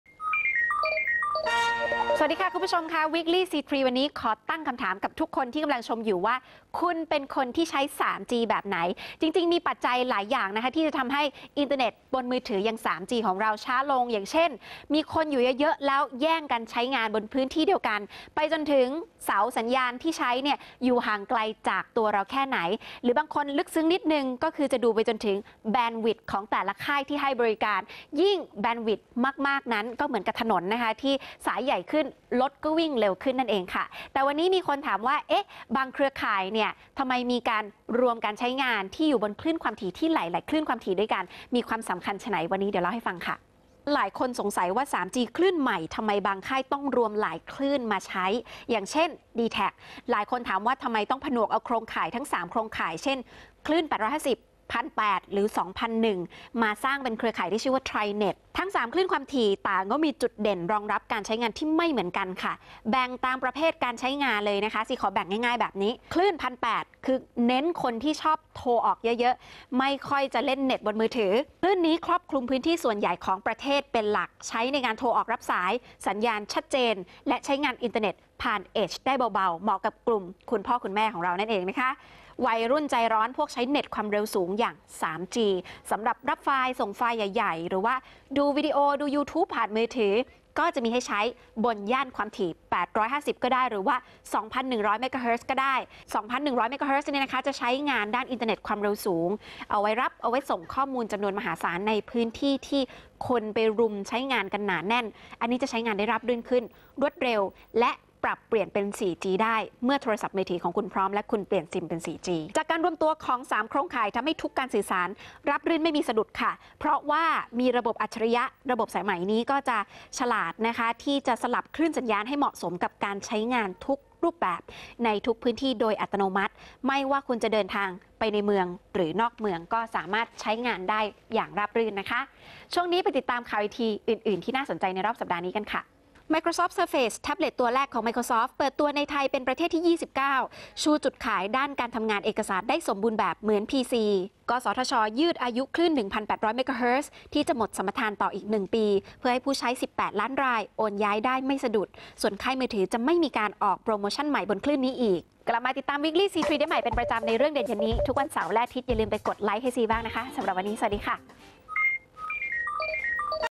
TriNet
Weekly C3 - Trinet

ปัจจัยต่างๆมีผลทำให้ความเร็วอินเตอร์ 3G นั้นจะเร็วหรือจะช้า หลายคนสงสัยว่าบางค่ายผู้ให้บริการ 3G นั้นทำไมมีหลายคลื่นความถี่ และแตกต่างกันอย่างไร ?
ติดตาม Weekly C3 ย้อนหลังผ่านช่องทางนี้